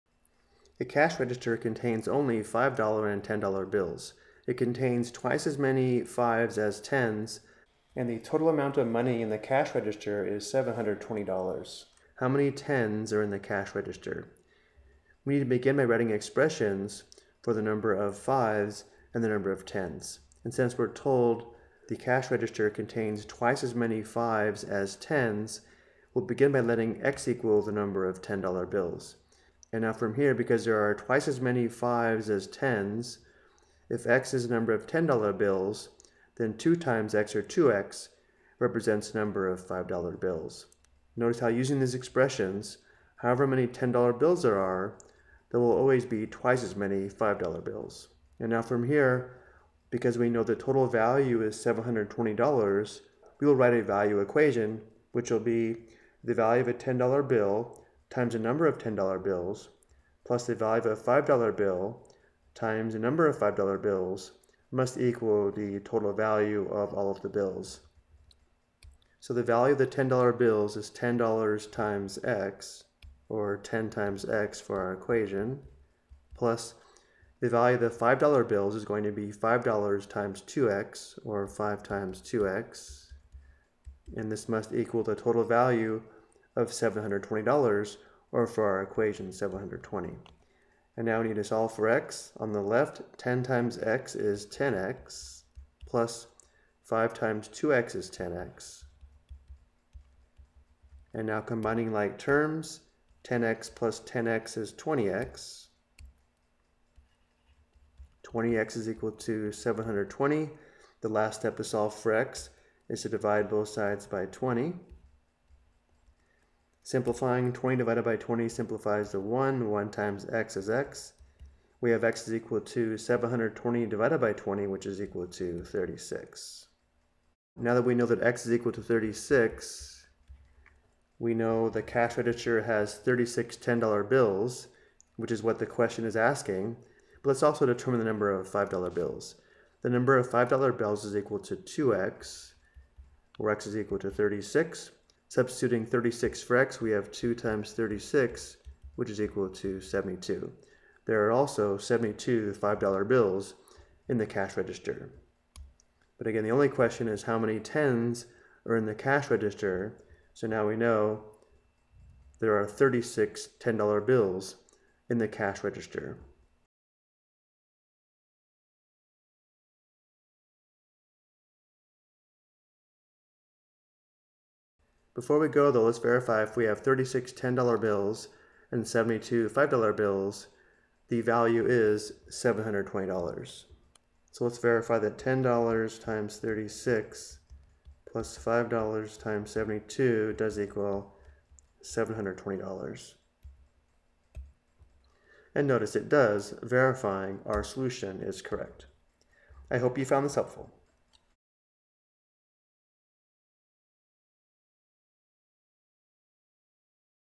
Mixture App: Number of 5's and 10's Given Total Value and Relationship (One Equation in One Var)
This video provides an example of how to solve a mixture problem using one equation in one variable.